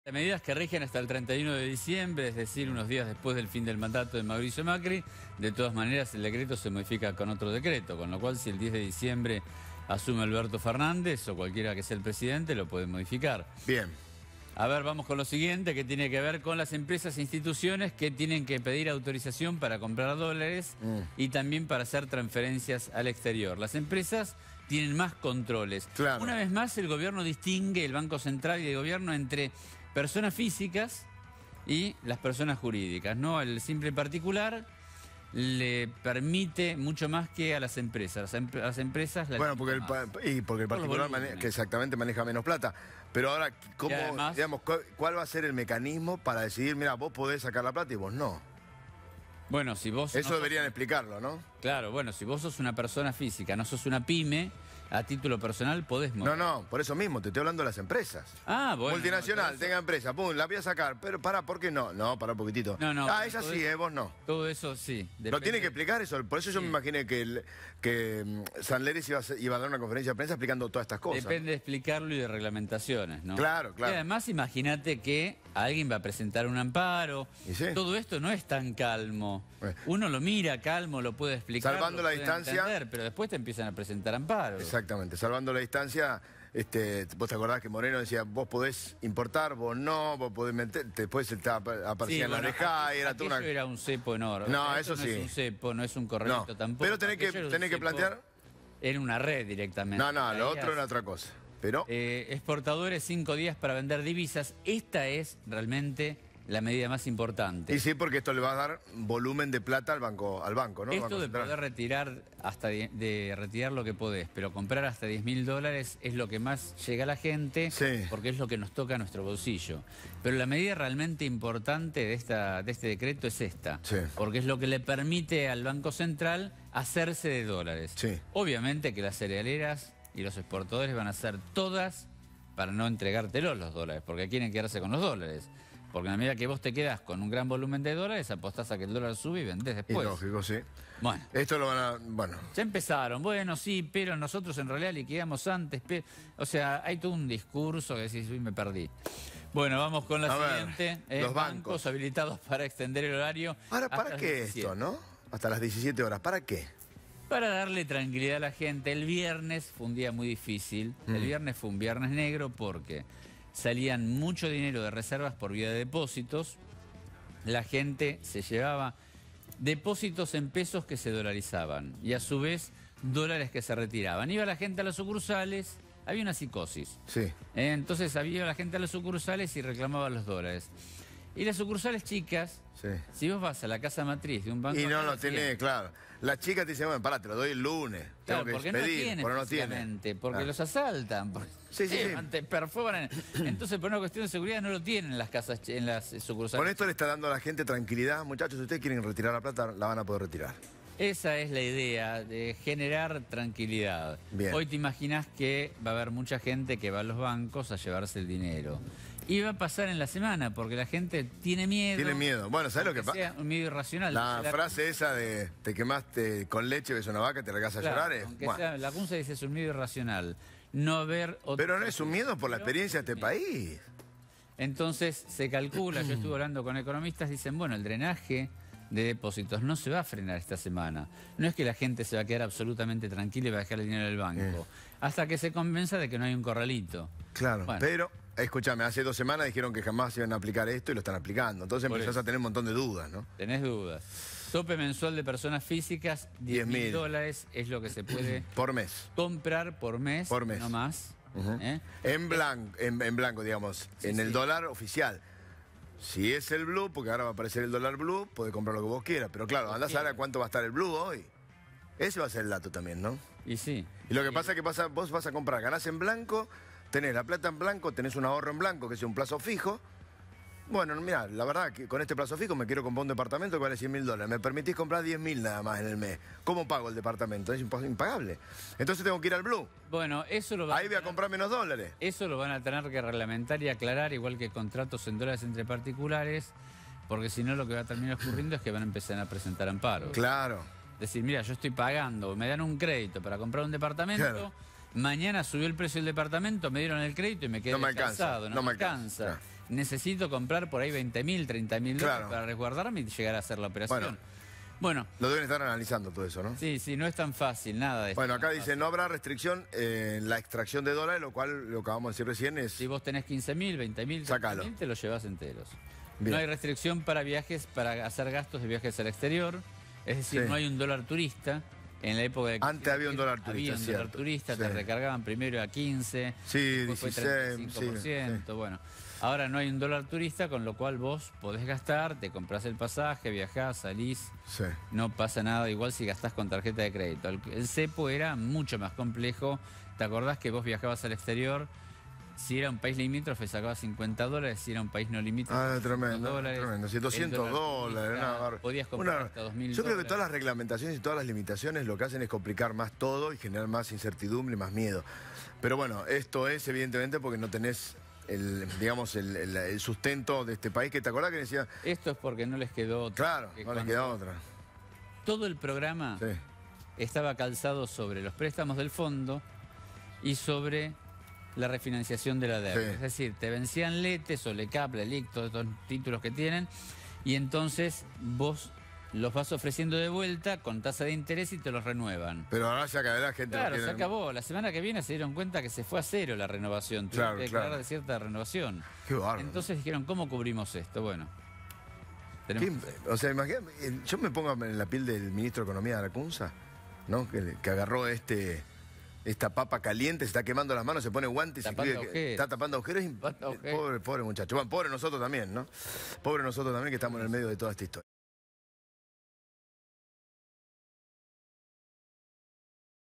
Rige el control de cambios para grandes operadores: las claves
Las claves de las medidas que limitan la compra de dólares. Hay un tope de US$ 10.000 por mes para las personas jurídicas. Desde el Gobierno advierten que son medidas para proteger el mercado cambiario y a los pequeños ahorristas. Los bancos podrían extender su horario de atención al público hasta las 17:00.
Ventura: "La idea es generar tranquilidad".